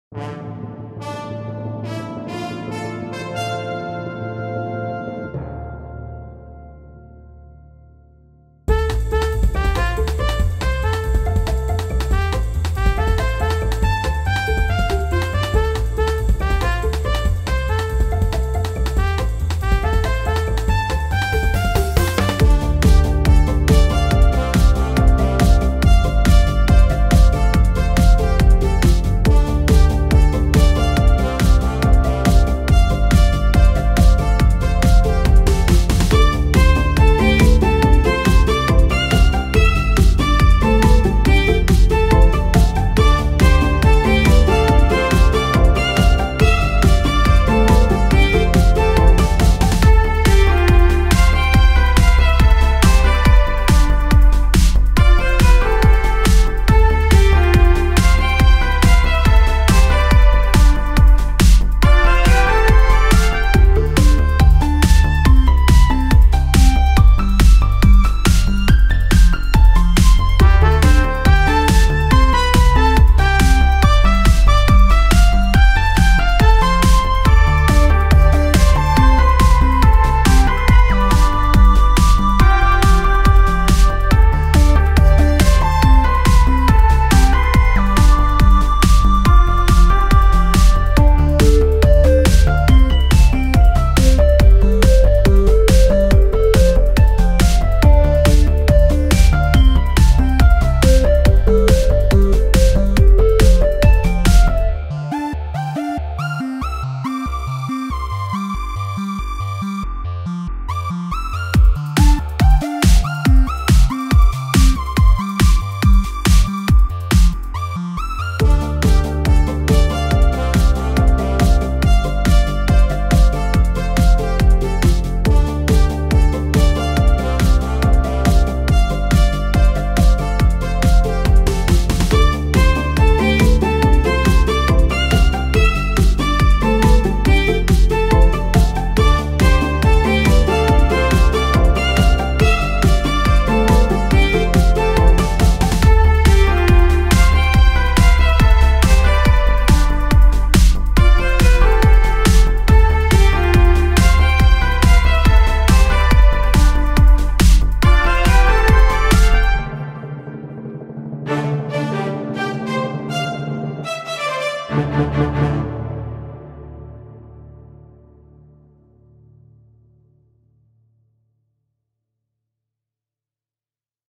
Jxudo World
Tried doing a bit a 3D Platformer vibe this time! Lemme know those brain nuggets!